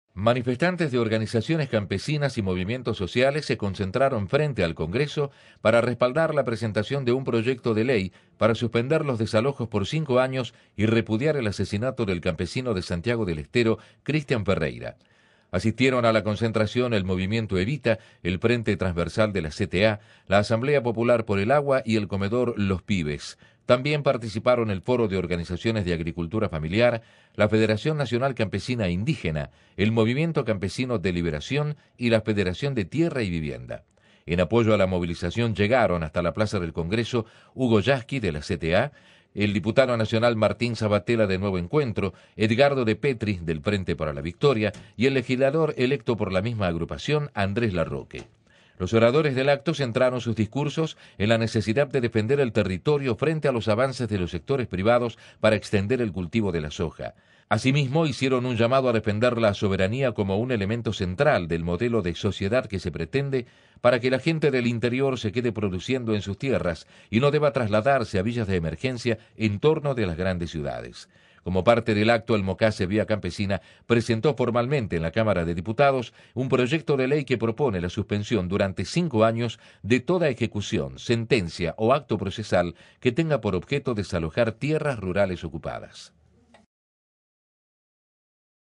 Organizaciones sociales se reunieron frente al Congreso para apoyar el reclamo de campesinos
Manifestantes de organizaciones campesinas y de movimientos sociales se concentraron frente al Congreso para respaldar la presentación de un proyecto de ley para suspender los desalojos por cinco años y repudiar el asesinato del campesino de Santiago del Estero Cristian Ferreyra.

Asistieron a la concentración el Movimiento Evita, el Frente Transversal de la CTA, la Asamblea Popular por el Agua, y el Comedor Los Pibes, además de representantes del Movimiento Nacional Campesino Indígena.

También participaron el Foro de Organizaciones de la Agricultura Familiar, la Federación Nacional Campesina Indígena, el Movimiento Campesino de Liberación, el Partido Comunista y la Federación Tierra y Vivienda, entre otros.

En apoyo a la movilización llegaron hasta la Plaza del Congreso Hugo Yasky, de la CTA; los diputados nacionales Martin Sabatella(Nuevo Encuentro), Edgardo Depetri (FpV), Diana Conti (FPV), Carlos Heller (Nuevo Encuentro) y el legislador electo Andrés Larroque (FPV).

Uno de los principales oradores del acto fue Adolfo Farías, del Mocase-Vía Campesina de Quimilí, quien llamó a "defender el territorio" al justificar que "para seguir avanzando con las hectáreas de soja, el único camino que tienen es el desalojo de los campesinos".

"Hay que cuidar la soberanía en algo que tiene que ver con el modelo de sociedad que queremos y que es a la gente del interior produciendo en sus tierras, y no trasladándose a villas miserias en torno de los grandes centros urbanos", explicó.

Y enfatizó: "Queremos que esos compañeros trabajen en el campo, para evitar que el `paco` sea el fin de sus historias".

Por su parte Ramiro Fresneda, del MNCI, dijo que "es necesaria la aprobación de la ley de tierras para evitar el desalojo de familias campesinas y el avasallamiento por parte del agronegocio".

Los convocantes pidieron que "el Poder Judicial pueda entender la posición histórica de las comunidades campesinas que no tienen títulos -de propiedad- contra la de los que tienen títulos y los usan como herramienta de expulsión y de violación a los derechos humanos".

Como parte del acto, el Mocase-VC presentó en la Cámara de Diputados un proyecto de ley que propone la suspensión por cinco años de "toda ejecución, sentencia, actos procesales o administrativos que tengan por objeto desalojar tierras rurales ocupadas".

La iniciativa fue presentada en Mesa de Entradas a las 14.30, y cuenta con la firma de los diputados Conti, Heller, Depetri, Sabbatella e Ines Pilatti (FPV).
Están presentes el Movimiento Evita, el Frente Transversal de la CTA, la Asamblea Popular por el Agua, y el Comedor Los Pibes, además de representantes del Movimiento Nacional Campesino
Indígena.
Participan de la marcha Hugo Yasky, de la CTA; los diputados nacionales Martin Sabatella(Nuevo Encuentro) y Edgardo Depetri (FpV); y Adolfo Farías, del Mocase-Vía Campesina de Quimilí, entre otros dirigentes.